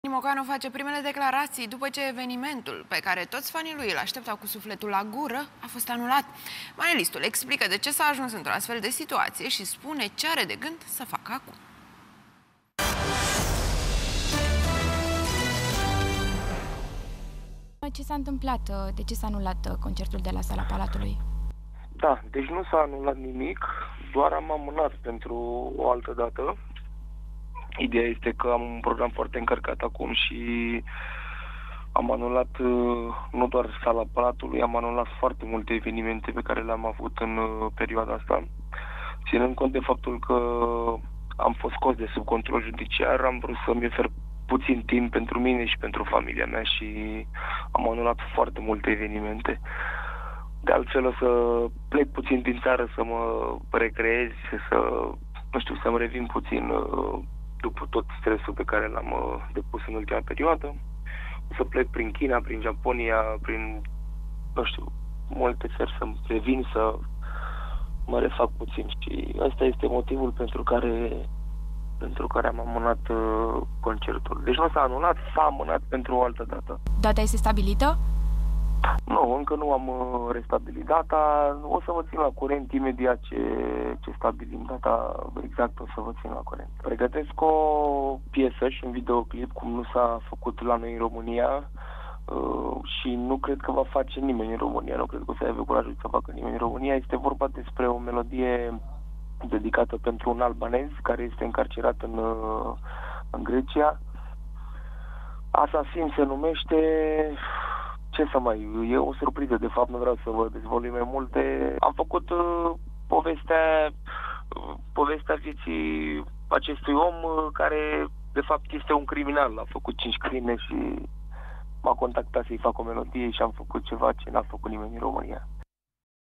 De ce i-a fost anulat concertul lui Dani Mocanu de la Sala Palatului
Dani Mocanu face primele declaraţii, după ce evenimentul pe care toţi fanii lui îl aşteptau cu sufletul la gură a fost anulat. Manelistul explică de ce s-a ajuns într-o astfel de situaţie şi spune ce are de gând să facă acum.